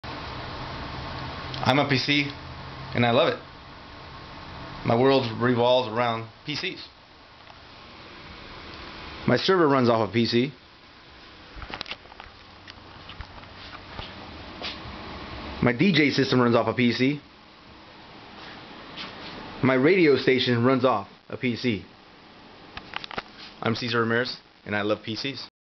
Cesar Ramirez Love's PCs
I'm Cesar Ramirez and I love PC since 1989 on a 286.